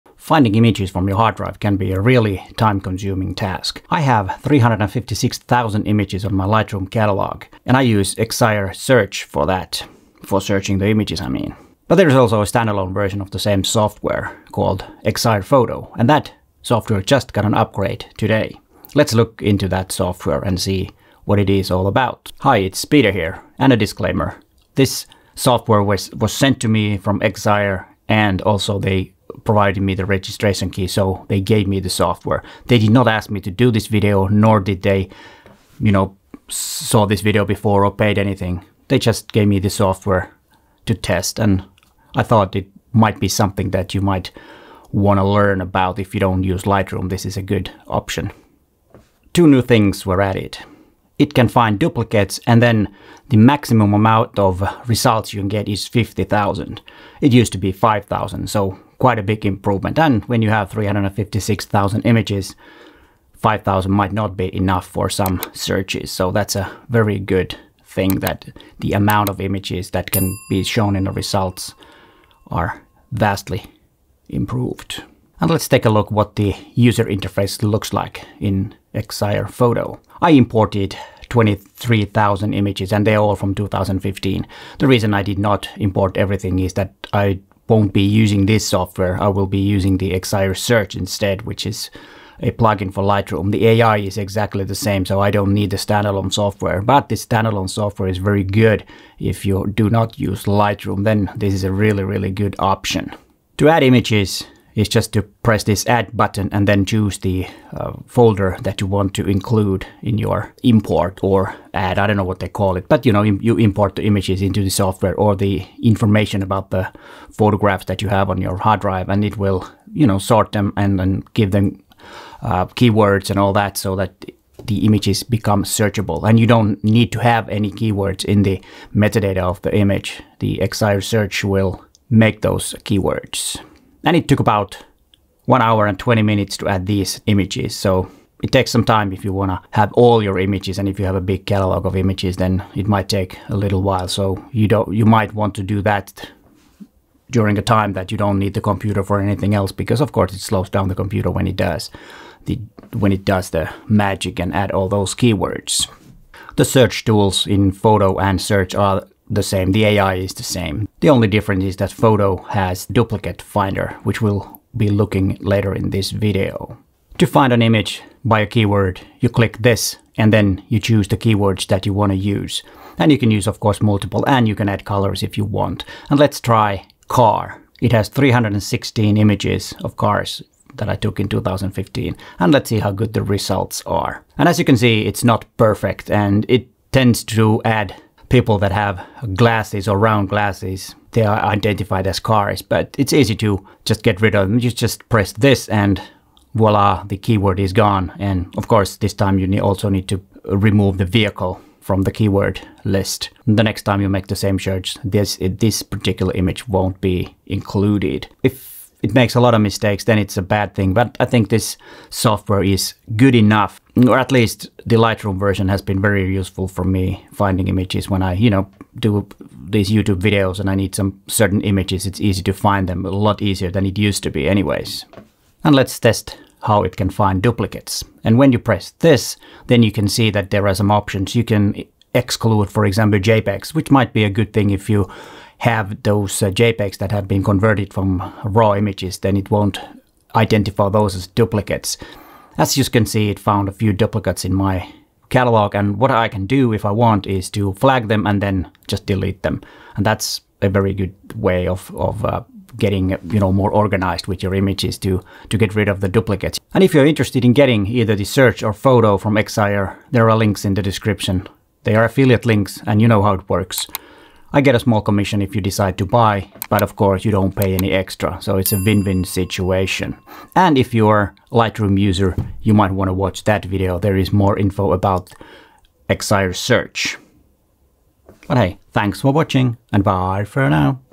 Cannot find your images? Try this 👉 Excire Foto
Excire Foto and Search are great AI-based search engines for you, if you have a lot of images finding them can be hard.

My Channel membership is now open!

⏱Timestamps
00:00 Intro
00:24 Disclaimer
00:49 What is new?
01:15 Excire Foto UI
01:43 Excire Foto UI: How to add images?
02:36 Excire Foto UI: Keyword Search Tool
03:46 Excire Foto UI: How to find duplicates?
04:16 Links to Excire Foto & Search
04:32 Watch Next!

You do not pay anything extra. Win-win.

► Want to learn more about photography or use your Olympus or OM SYSTEM  camera.